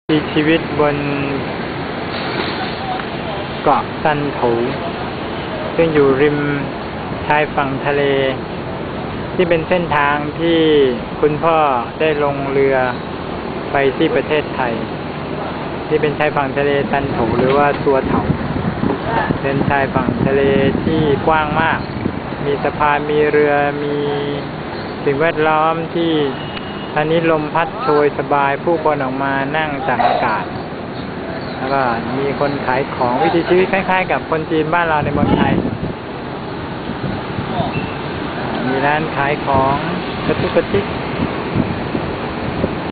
(จีน)ตามรอยเท้าพ่อ(บังเกิดเกล้า)ที่ซ่านโถว.สวนริมท่าเรือ ตอน 2
ซัวเถา เป็นหนึ่งในจังหวัดของจีนที่ตั้งอยู่ในมณฑลกวางตุ้ง มีพื้นที่ 234 ตารางกิโลเมตร จังหวัดซัวเถา ตั้งอยู่ทางทิศตะวันออกเฉียงเหนือของมณฑลกวางตุ้ง ซัวเถาเป็นหนึ่งในถิ่นฐานของจีนแต้จิ๋ว ที่พูดภาษาแต้จิ๋ว ซึ่งเป็นภาษาที่แยกย่อยมาจากภาษาหมินใต้(ฮกเกี้ยนใต้) เมืองซัวเถา ในสมัยราชวงศ์ราชวงศ์ซ้อง เคยเป็นเมืองท่าของ เมืองถัวเจียง และ อำเภอเจียหยาง มีชื่อว่า เมืองซ่าหลิ่ง ในสมัยราชวงศ์หยวน ในปีพ.ศ. 2106 เมืองซัวเถาเป็นส่วนหนึ่งของอำเภอเฉิงไห่ หรือเถ่งไฮ่ ในภาษาแต้จิ๋ว จังหวัดฉาวโจวหรือจังหวัดแต้จิ๋วในปัจจุบัน ในตอนนั้นเมืองซัวเถาถูกเรียกว่า ชาชานปิง ส่วนคำว่าซัวเถานั้นถูกเรียกเมื่อคริศตศตวรรษที่ 17 จากเครื่องชามที่เรียกว่า ชาชาน โทวเปาไท และก็ได้มาเป็นเมืองเมื่อ พ.ศ. 2452